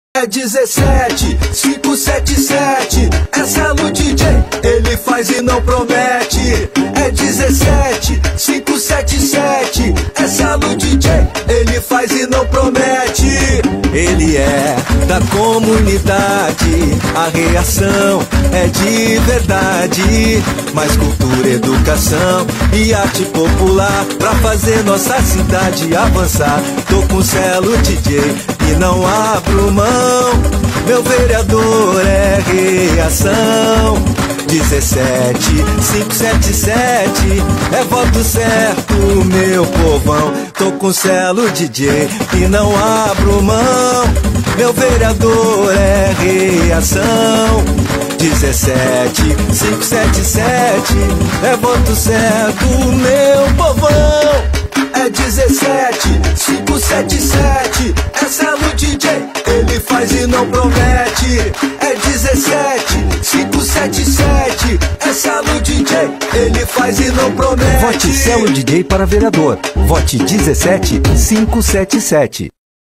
Clipe Celo DJ
Clipe do Jingle do candidato a Vereador Celo DJ (PSL)
Cidade: Porto Alegre
Eleições Municipais 2020